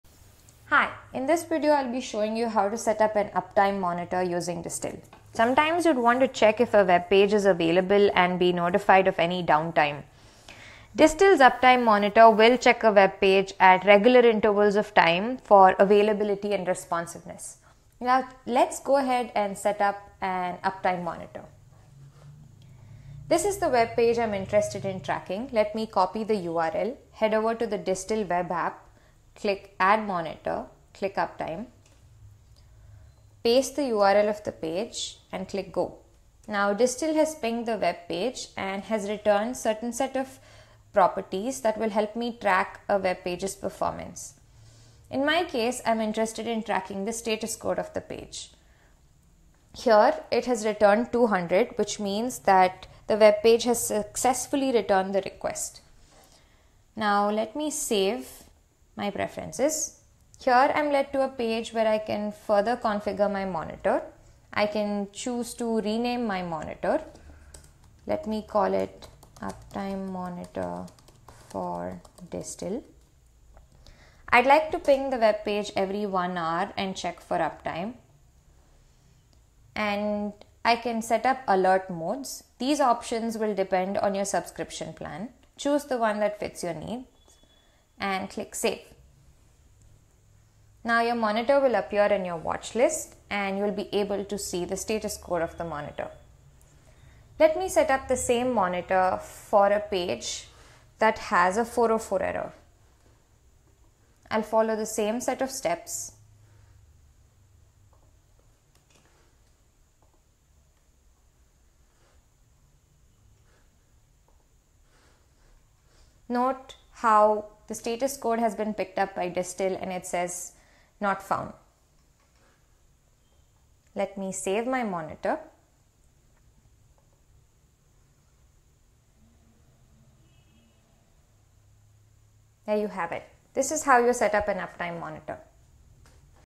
Uptime Monitoring using Distill
Distill's uptime monitor periodically sends requests to the webpage, returns the response, and notifies you of changes in the webpage properties. This video walks you through the steps to set up an uptime monitor that can help you track the status code and other header properties of the webpage.